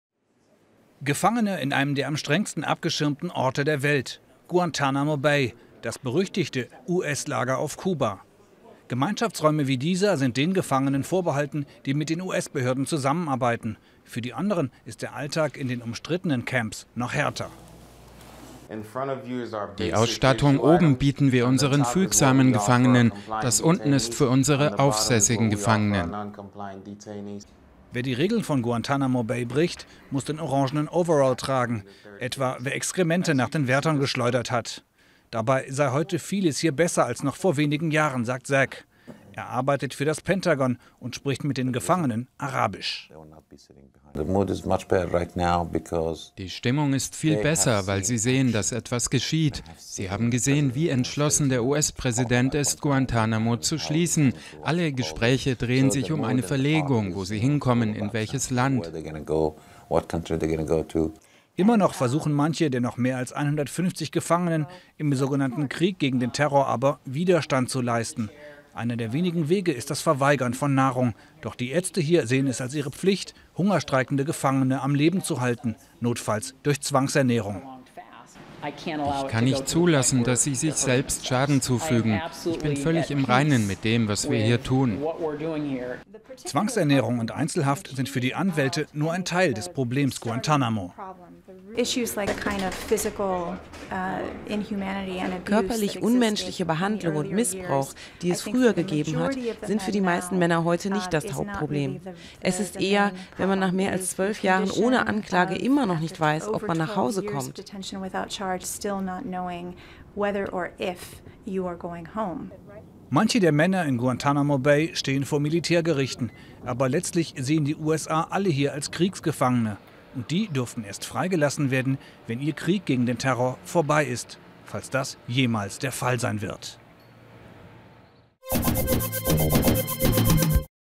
Strengstens abgeschirmt: Gefangen in Guantanamo
Das US-Gefangenenlager Guantanamo Bay auf Kuba ist einer der am strengsten abgeschirmten Orte der Welt - AFP-Reporter konnten einen Blick hinter den Stacheldraht werfen. Die Lage der noch mehr als 150 Gefangenen im sogenannten Krieg gegen den Terror hat sich in den vergangenen Jahren verbessert, viele sitzen hier aber weiter ohne Anklage und ohne Aussicht auf einen Prozess ein.